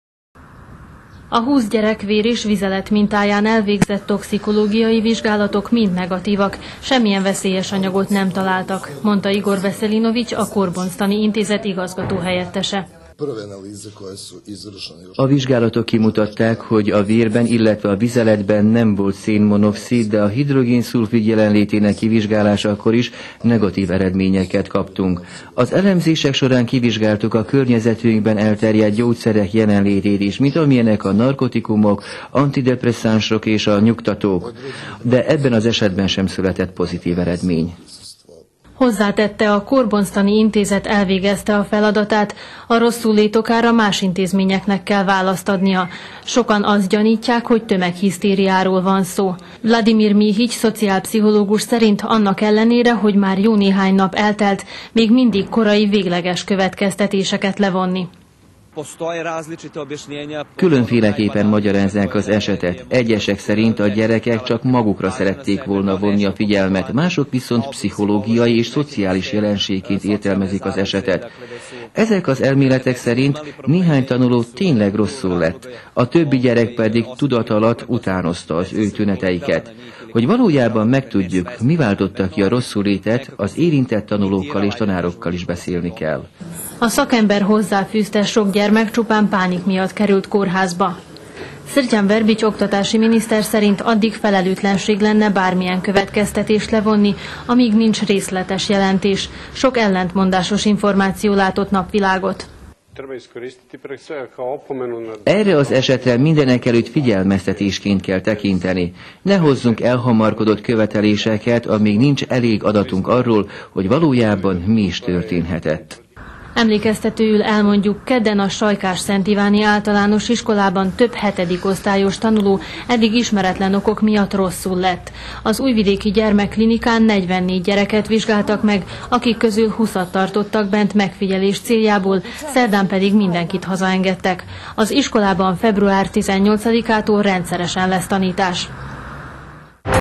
Nem volt mérgezés
A sajkásszentiváni általános iskolában történt tömeges rosszullét nagy figyelmet keltett az elmúlt napokban, ennek ellenére még mindig nem tudhatjuk biztosan, hogy mi is történt valójában. Mindenesetre a  tokszikológiai vizsgálatok eredményei negatívak.